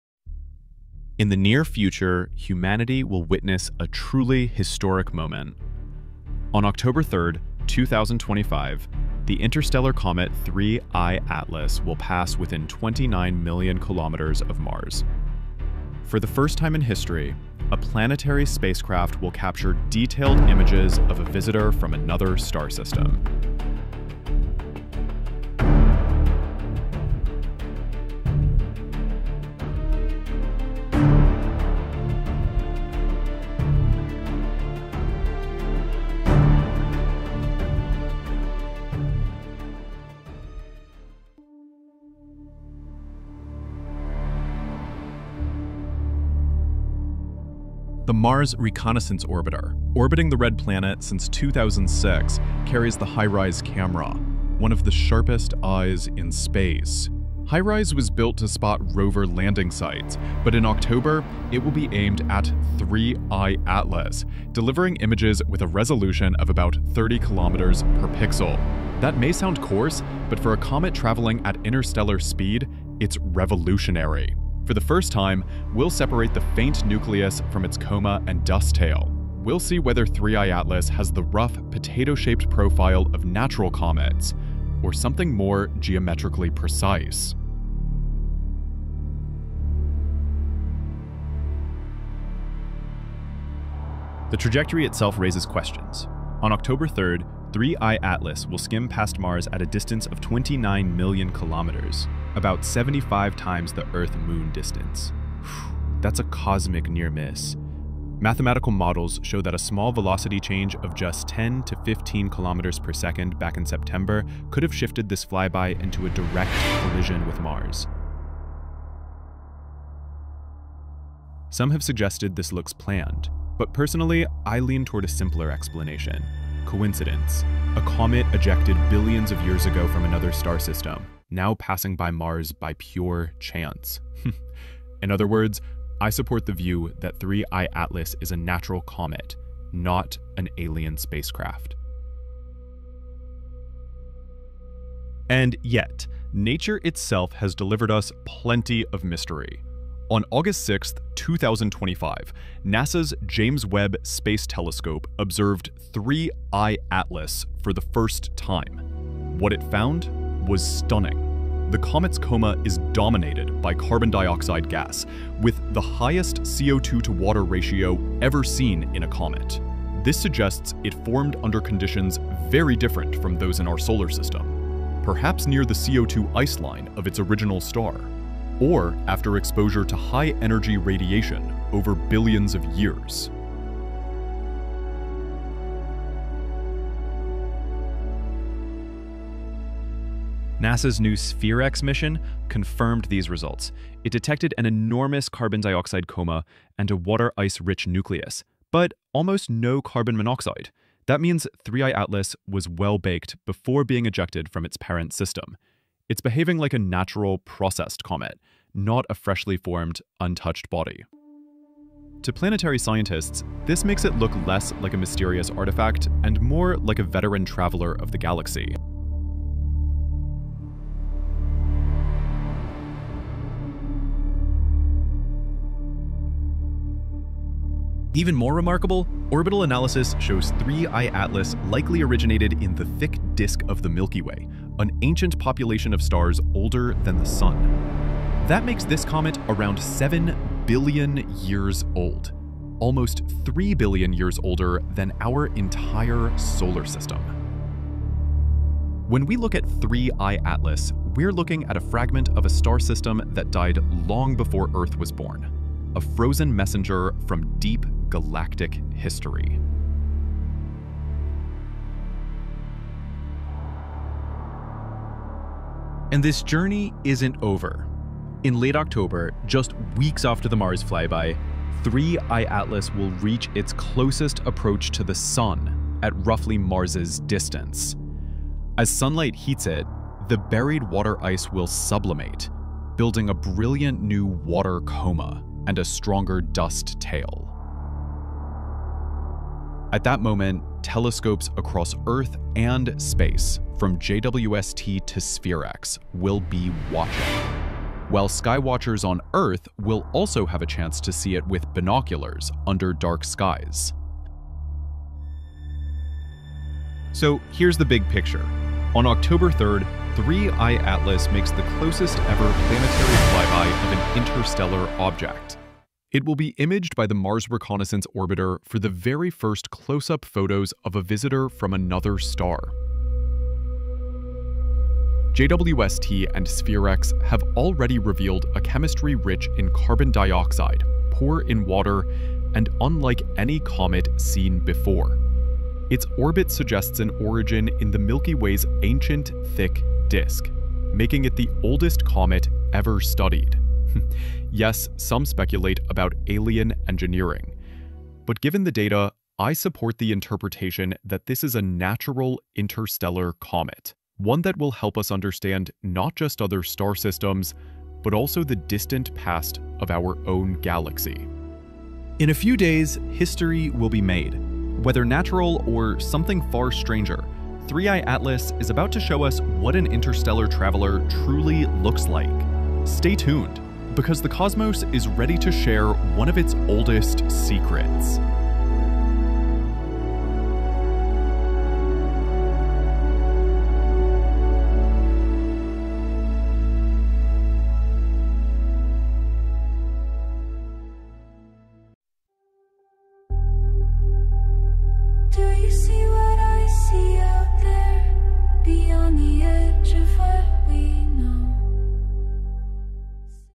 Historic Mars Encounter: 3I/ATLAS Comet’s Unprecedented Mars Flyby
🚀 **21 Days Until History: Interstellar Comet 3I/ATLAS Meets Mars** 🌌

On **October 3rd, 2025**, the interstellar comet **3I/ATLAS** will make a **historic flyby of Mars**, passing just **29 million km** from the Red Planet. For the first time in human history, we’ll have spacecraft like **NASA’s Mars Reconnaissance Orbiter** ready to capture **close-up images of an object from another star system**.

🔭 The **James Webb Space Telescope (JWST)** has already taken a **first look at 3I/ATLAS**, uncovering something no one expected: its **coma is dominated by carbon dioxide**, with the **highest CO₂-to-water ratio ever seen in a comet**. Combined with discoveries from **SPHEREx** and other observatories, this strange chemistry suggests 3I/ATLAS may have formed billions of years ago in the **thick disk of the Milky Way**, making it possibly the **oldest comet ever studied**.

In this video, we’ll explore:

* 🌠 Why 3I/ATLAS is unlike any comet we’ve seen before
* 📷 How **HiRISE** on Mars will photograph it at 30 km/pixel resolution
* 🧪 The shocking chemical makeup revealed by **JWST & SPHEREx**
* 🕵️ Theories about whether it’s a natural relic or something artificial
* 🪐 Why this flyby could reshape our understanding of **interstellar visitors**

Whether natural or artificial, **3I/ATLAS is a once-in-a-lifetime opportunity** to study an object born around another star. Join me as we count down to the **Mars encounter of the century.**

# 🔑 **Keywords**

# 🏷️ **Hashtags**